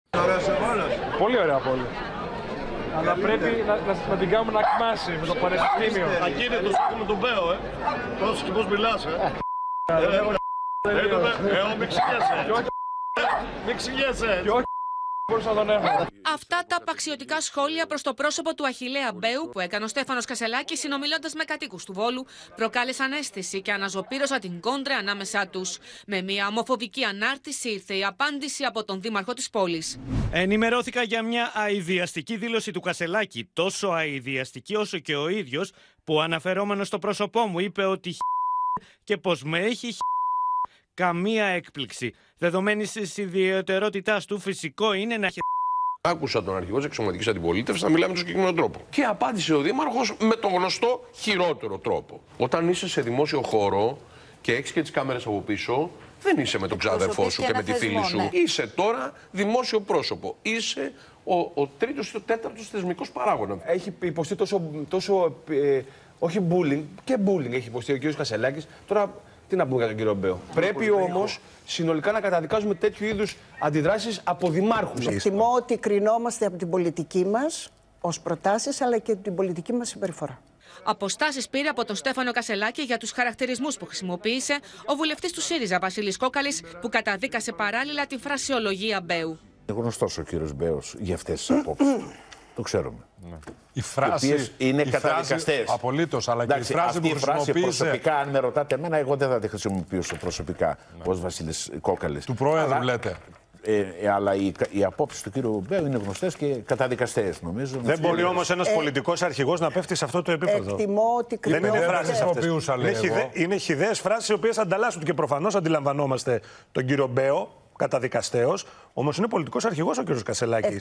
Απαξιωτικό σχόλιο Κασσελάκη κατά Μπέου | Μεσημεριανό Δελτίο Ειδήσεων 12/02/2024 | OPEN TV
Νέος γύρος σφοδρής αντιπαράθεσης άνοιξε ανάμεσα σε Στέφανο Κασσελάκη και Αχιλλέα Μπέο, με ακραίους χαρακτηρισμούς και ομοφοβικές απαντήσεις. Αφορμή στάθηκε η στιχομυθία που είχε ο πρόεδρος του ΣΥΡΙΖΑ με κατοίκους του Βόλου, όπου με απαξιωτικό τρόπο, μπροστά στις κάμερες, αναφέρθηκε στο πρόσωπο του δημάρχου της πόλης και τον Αχιλλέα Μπέο να επιστρέφει,  με προσβλητική ανάρτηση, τα πυρά στον Στέφανο Κασσελάκη.